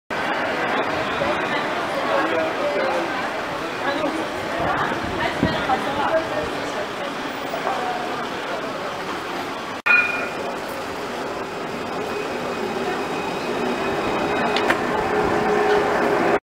Dublin Fire Brigade 'Reserve 5' Ambulance arriving on scene medical emergency in a supermarket
EN: Ambulance 'Reserve 5' of the Dublin Fire Brigade arriving on scene of a medical emergency in the city center

NL: Ambulance 'Reserve 5' van de Dublin Fire Brigade arriveert met spoed voor een medische noodsituatie in het centrum